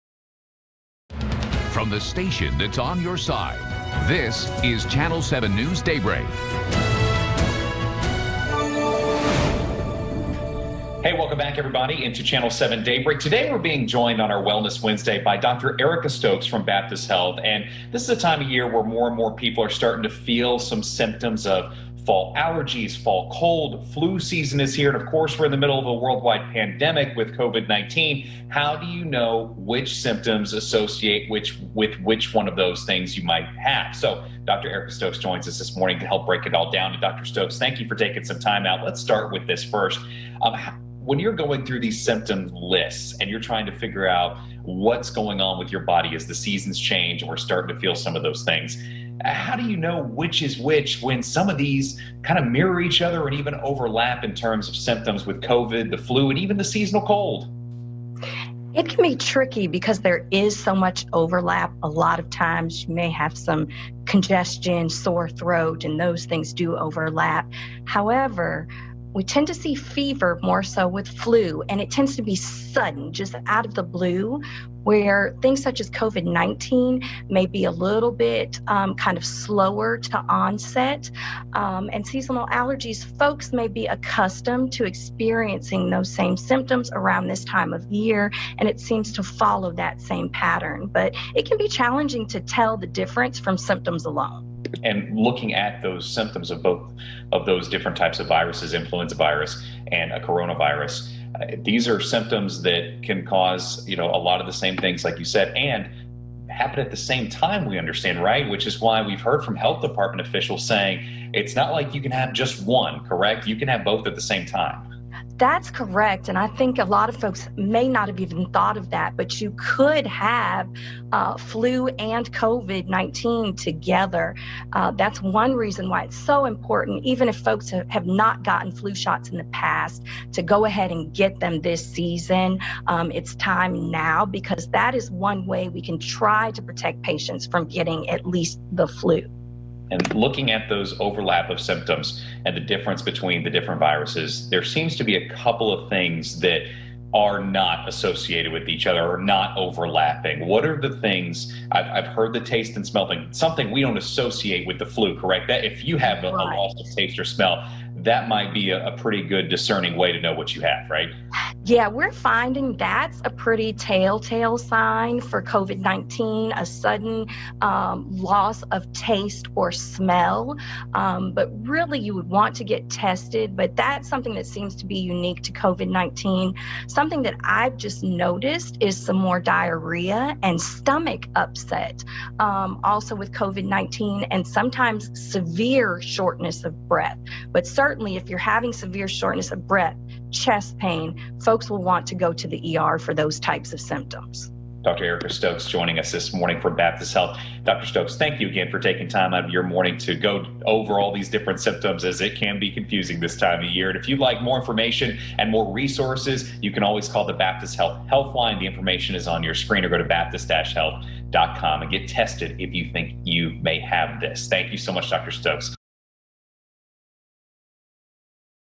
Dr. Erica Stokes with Baptist Health Discusses Similarities/Differences Between COVID-19, Flu & More
Dr. Erica Stokes, a family medicine doctor at Baptist Health Family Clinic-Otter Creek, talks about the symptoms most often seen in patients with COVID-19, flu, & seasonal allergies and how they overlap/differ from each other.

This story aired on @katvchannel7